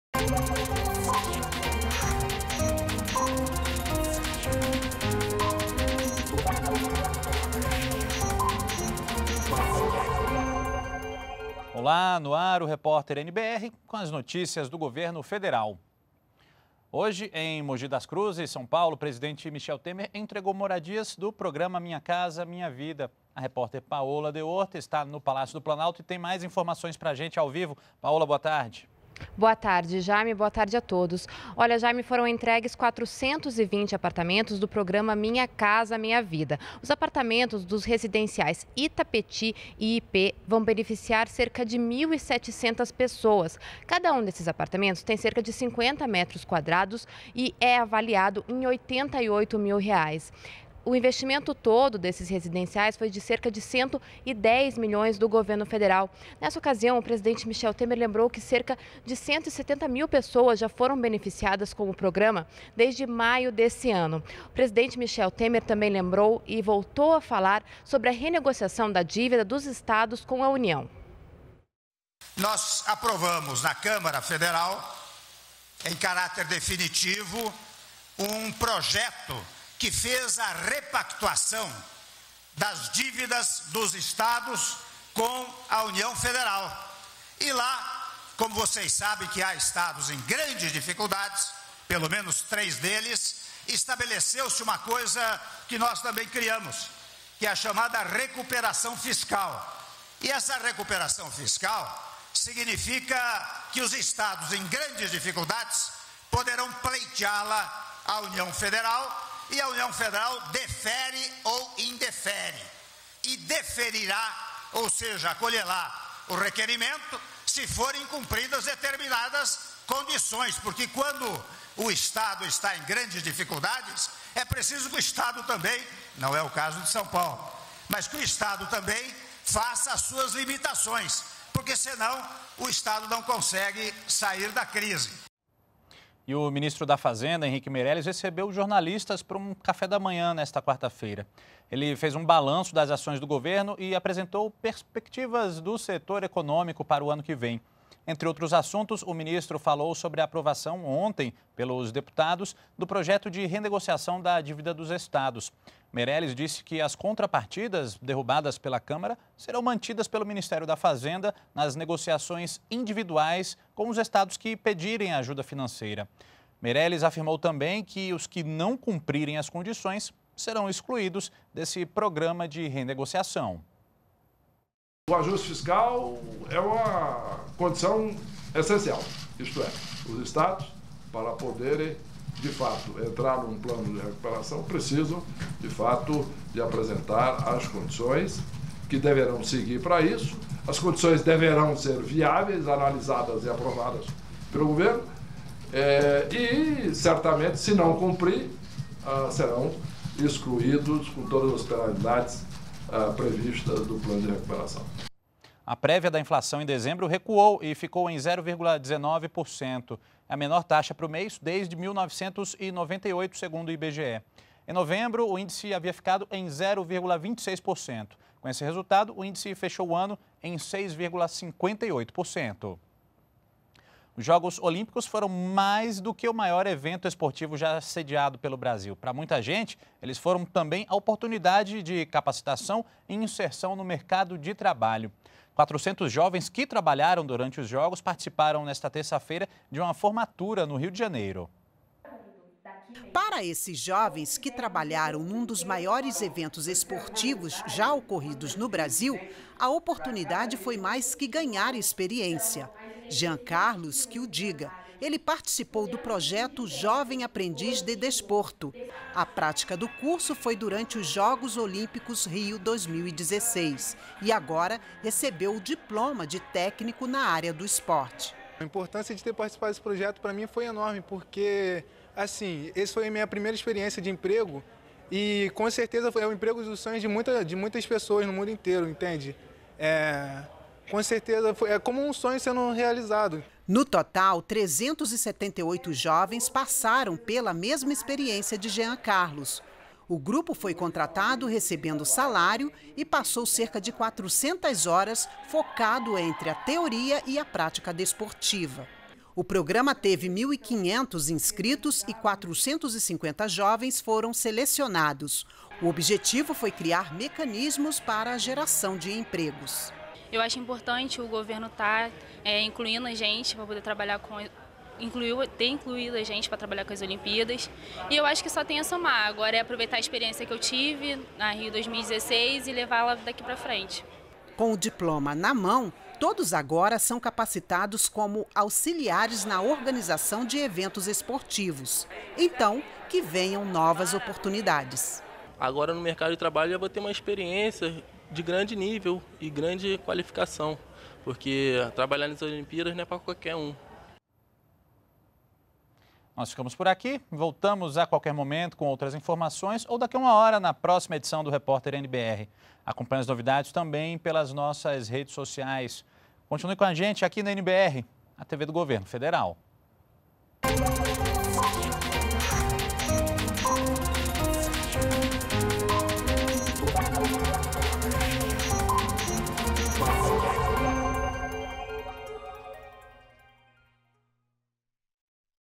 Minha Casa Minha Vida beneficiou 170 mil pessoas desde maio de 2016
REPÓRTER NBR 16H - 21.12.16: Presidente Michel Temer entrega 420 residências do Minha Casa Minha Vida em Mogi das Cruzes (SP). Em discurso, Temer lembrou que o programa habitacional já beneficiou 170 mil brasileiros desde maio deste ano. E mais: Ministro da Fazenda, Henrique Meirelles, conversou com jornalistas durante café da manhã. Meirelles fez um balanço das ações do governo e apresentou uma perspectiva do setor econômico para 2017. Veja também: prévia da inflação recuou em dezembro, ficando em 0,19%. É a menor taxa para o mês desde 1998.